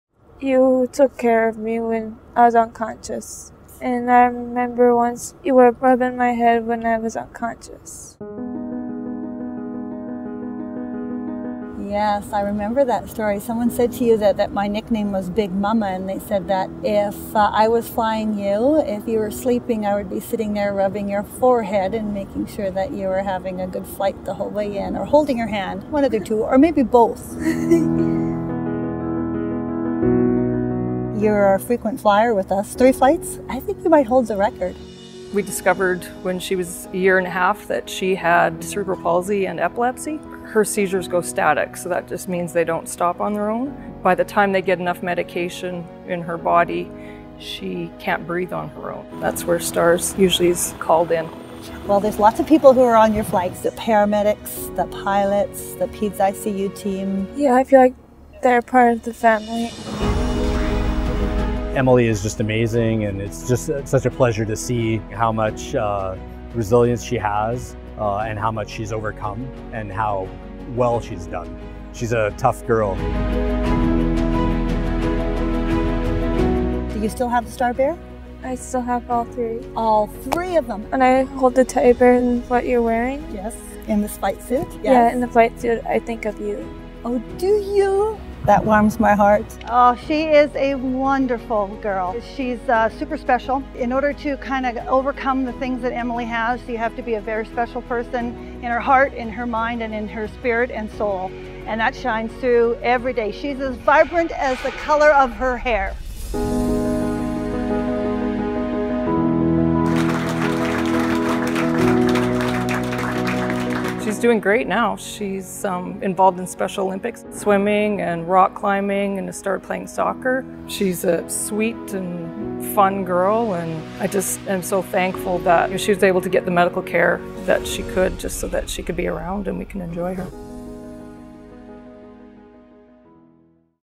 This three-time patient has a special bond with our crew
We flew Very Important Patient Emily Donohue three times when she was quite young, and now she's happy to be one of our greatest allies.